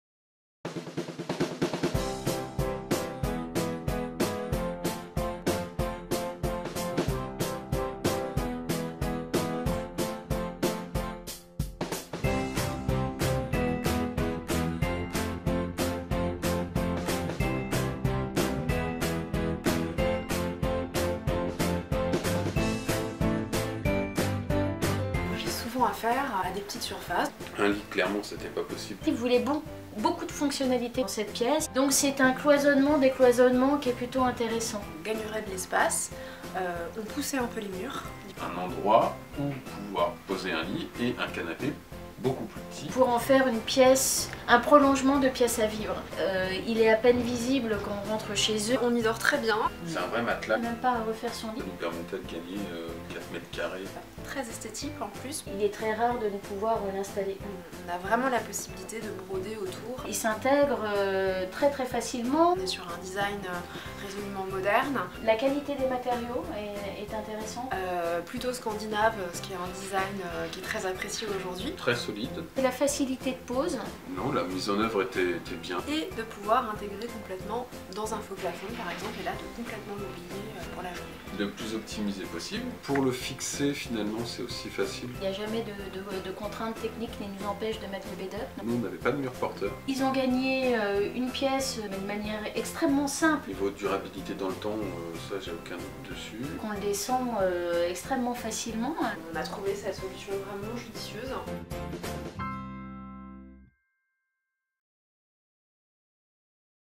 OBJECTIF GAIN DE PLACE : Trois architectes vous donnent leurs conseils
Découvrez bedup® le lit qui monte au plafond